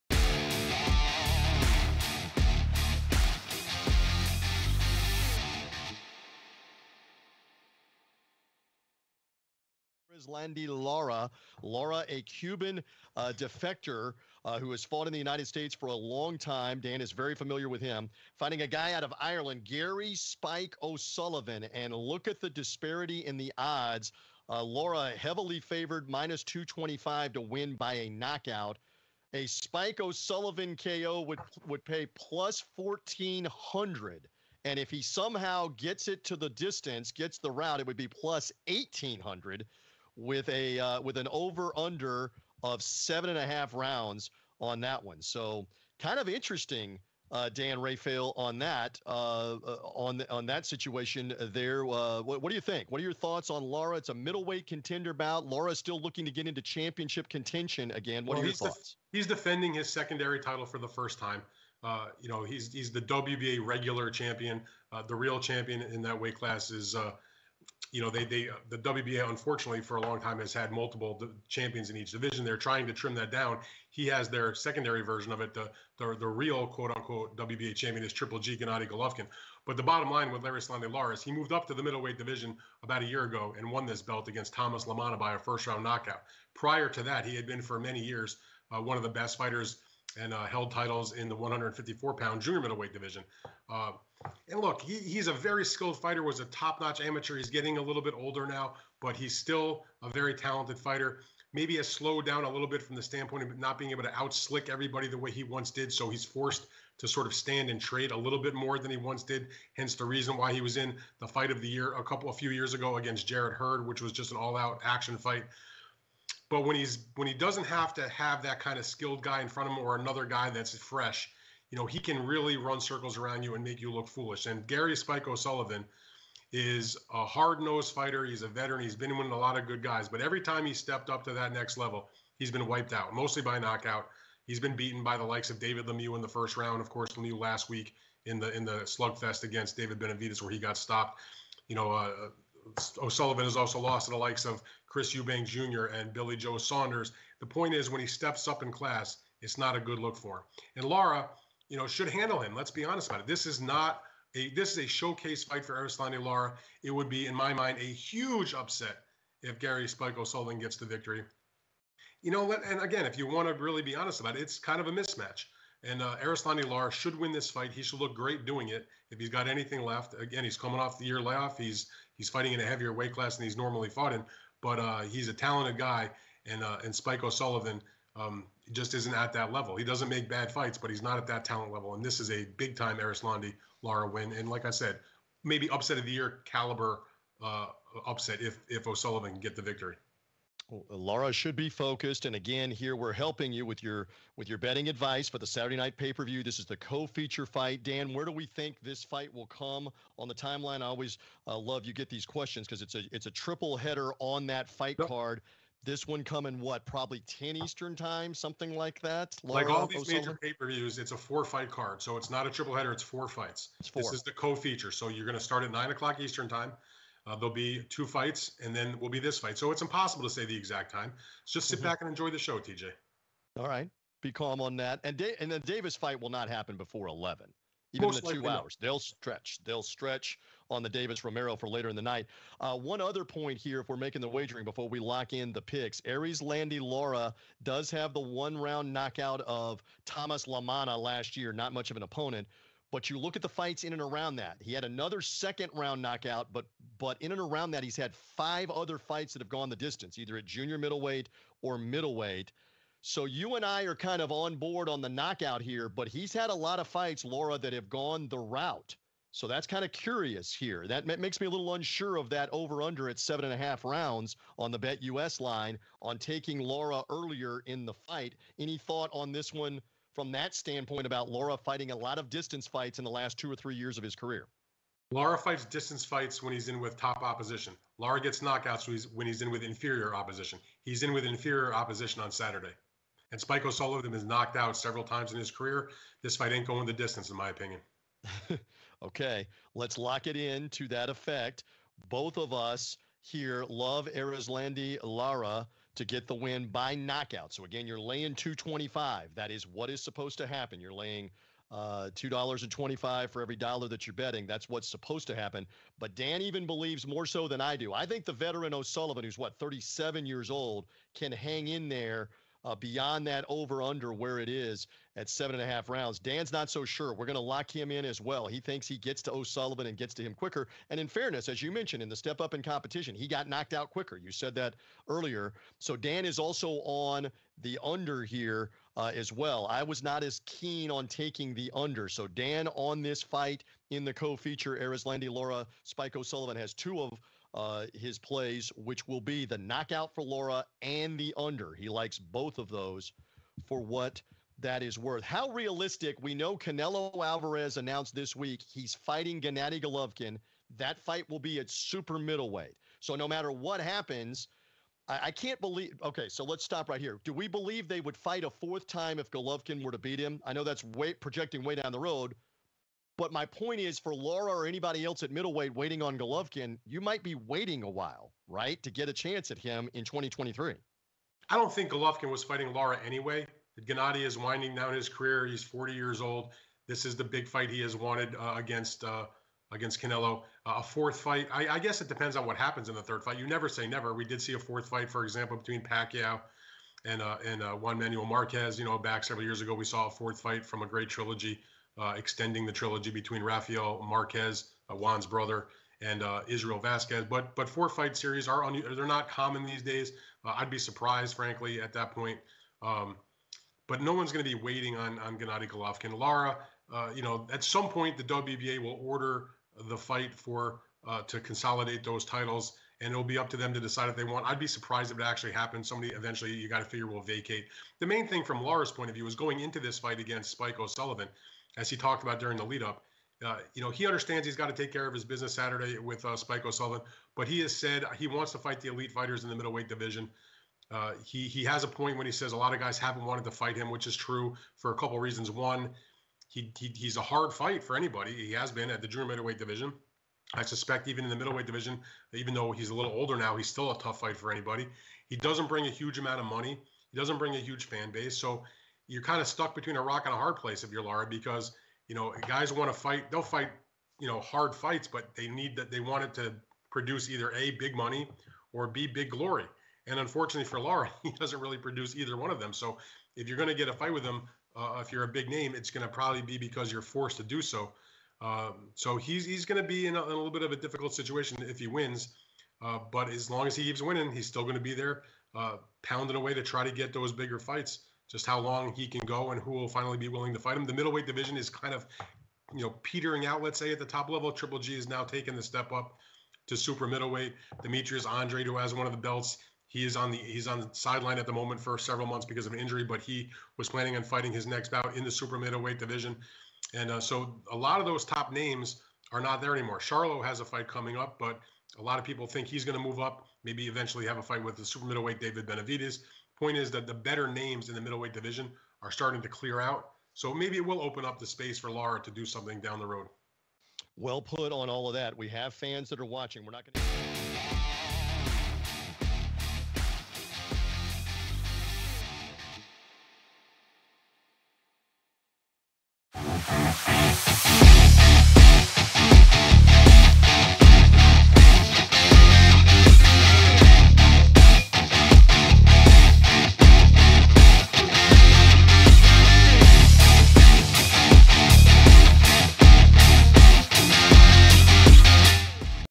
Erislandy Lara vs Spike O’Sullivan (5-28-22) Boxing Expert Predictions, Boxing Picks & Best Bets
Erislandy Lara vs Spike O’Sullivan May 28th 2022 Fight Preview.

The BetUS-TV boxing show takes a deep dive into Saturday’s middlweight bout between Erislandy Lara vs Spike O’Sullivan.

Lara comes in as a big favorite and looks to pound out another victory while climbing the ladder. He is 28-3-3 while O’Sullivan is 31-4. O’Sullivan is 37 so he is fighting for his professional career. There are only so many rounds left in you as you agre. Lara has three straight victories, the last in May 2021 over Tomas LaManna. Lara enters as a massive favorite.

Connect with our betting experts on Twitter:

Dan Raphael @DanRafael1

Erislandy Lara photo:
Rob Foldy / GETTY IMAGES NORTH AMERICA / AFP